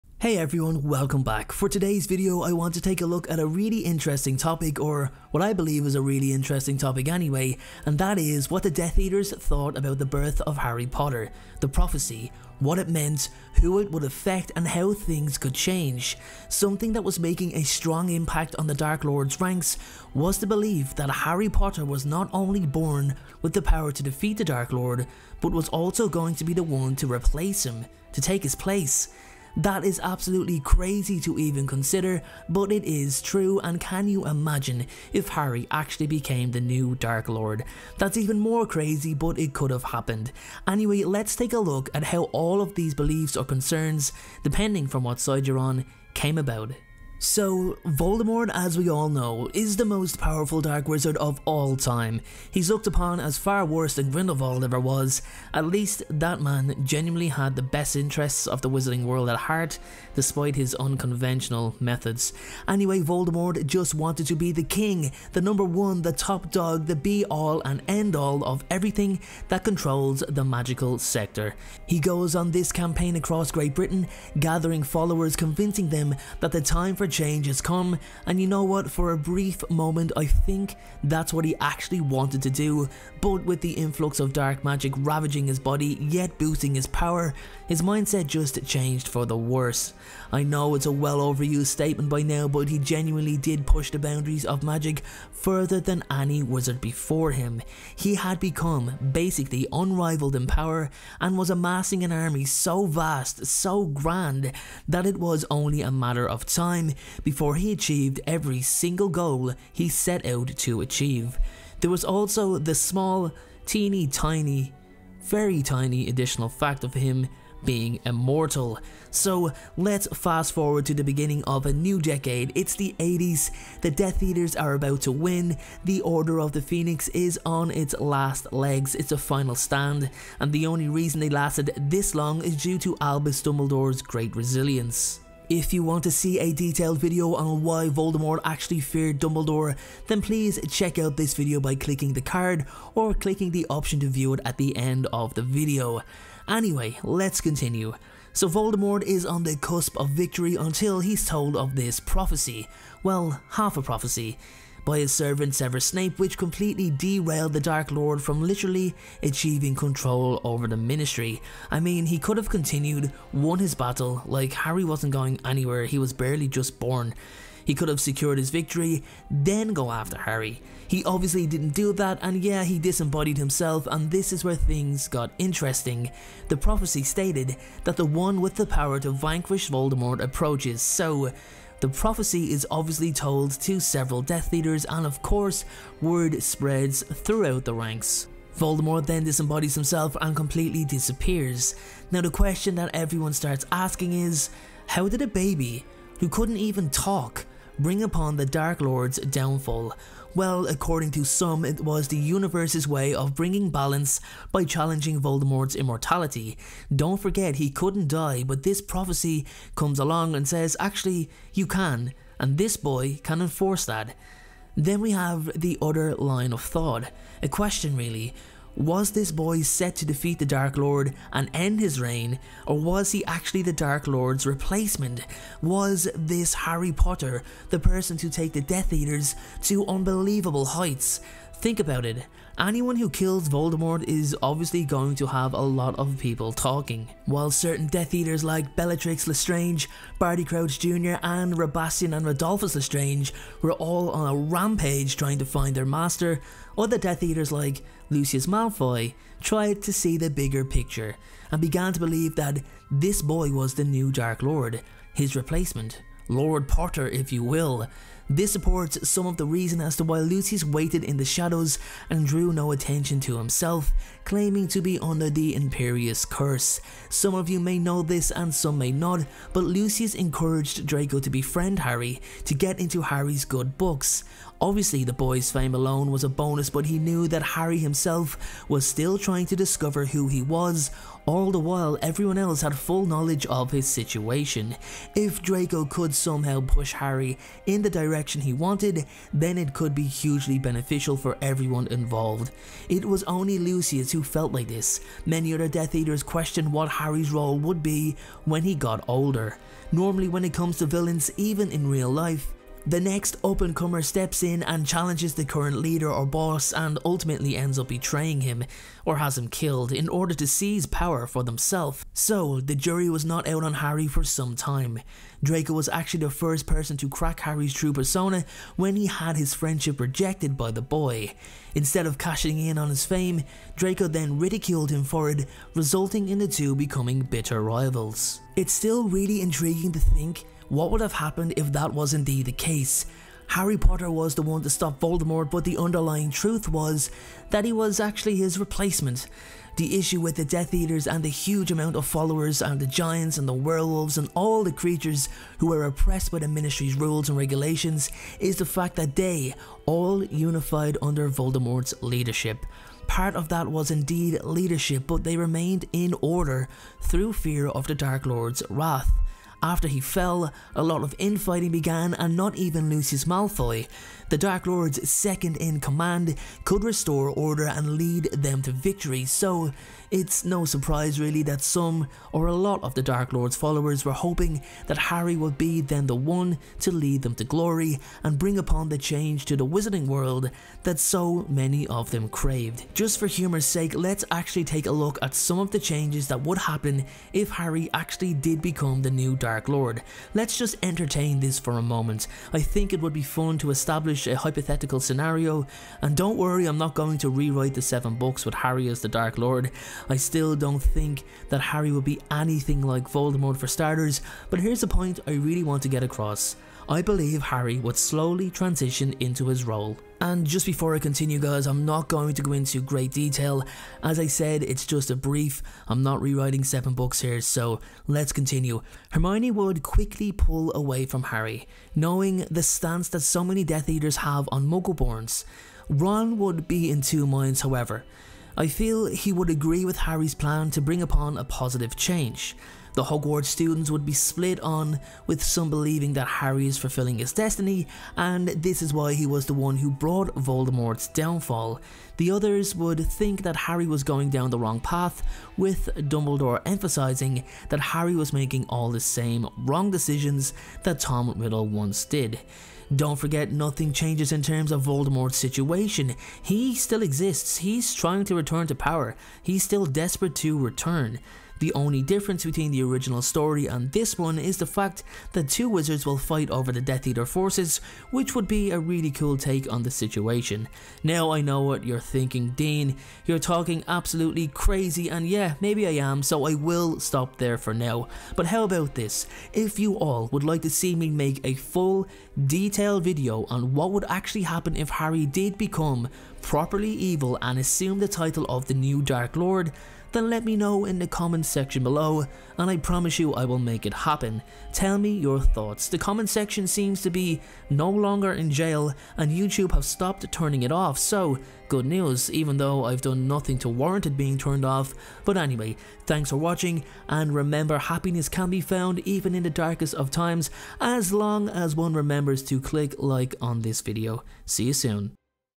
NEW - Why The Death Eaters Believed Harry Was The NEW Dark Lord
For todays video I want to take a look at what the death eaters thought about the birth of Harry Potter, the prophecy, what it meant, who it would effect and how things could change. Something that was making a strong impact on the dark lords ranks was the belief that Harry Potter was not only born with the power to defeat the dark lord but was also going to be the one to replace him. Can you imagine if harry actually was the new dark lord?